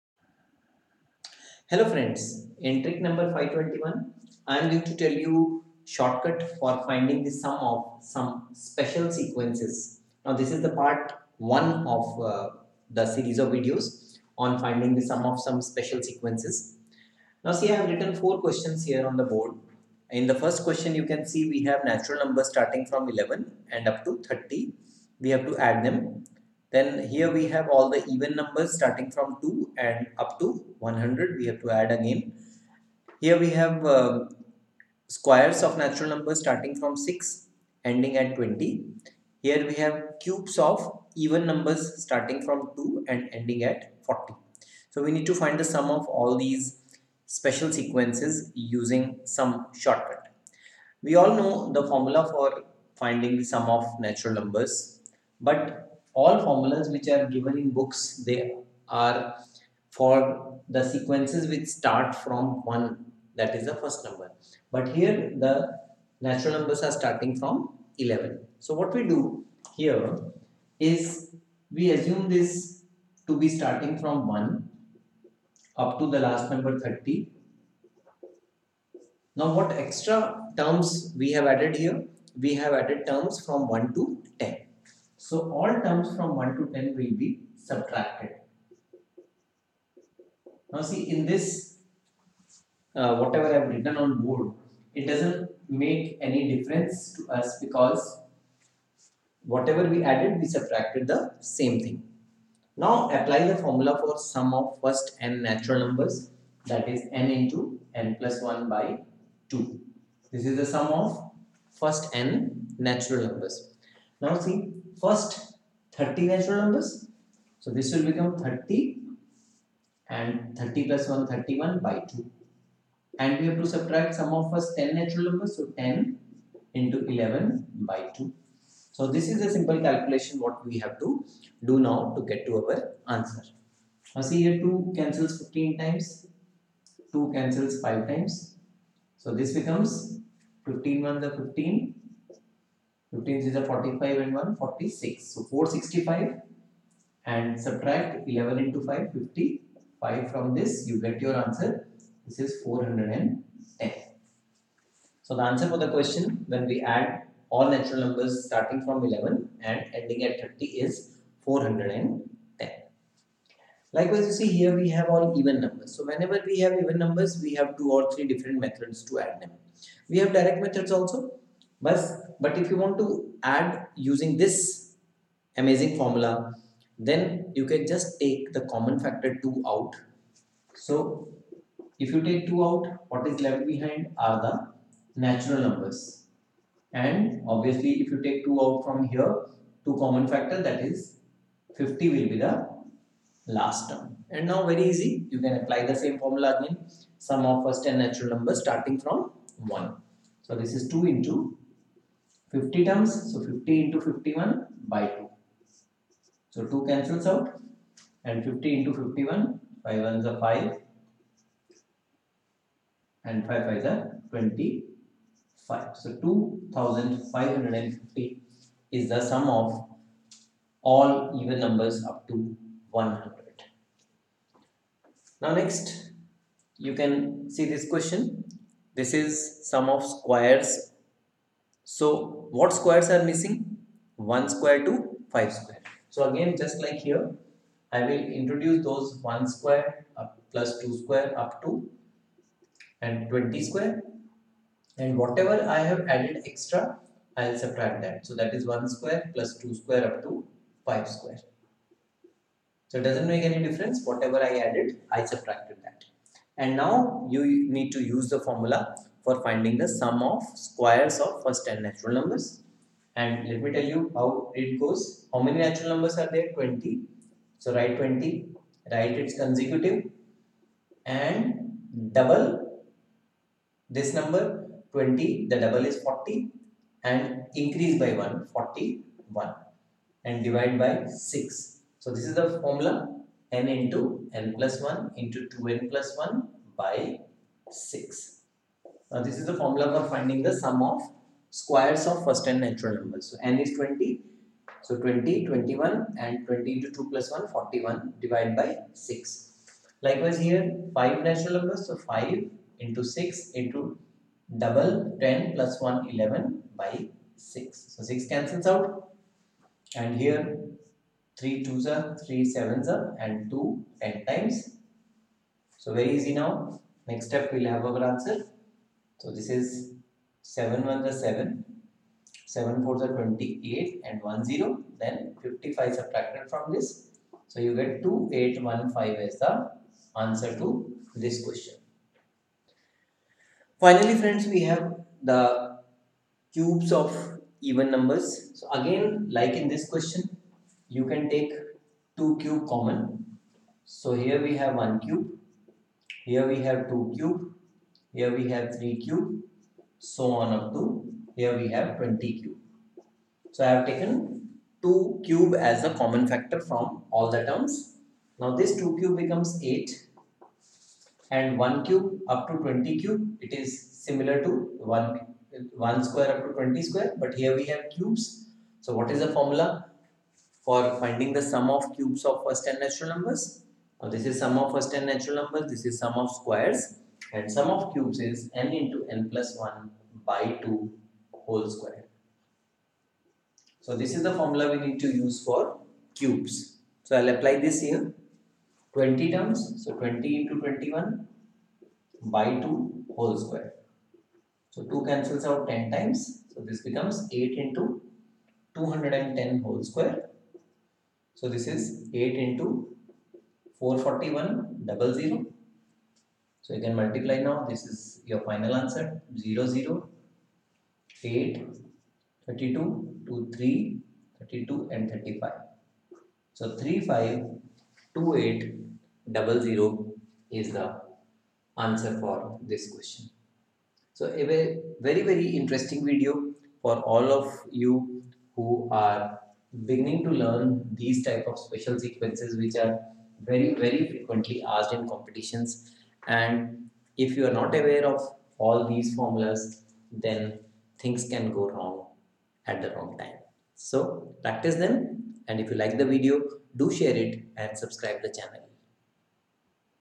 Trick 521 - Find Sum of Some Special Series Easily
Sum of first 'n' natural numbers, the sum of squares and sum of cubes. there are shortcuts available for finding these easily. but if the series does not start from 1, then there is a separate shortcut to solve such questions. MUST WATCH VIDEO FOR ALL COMPETITIONS. for more videos, keep visiting: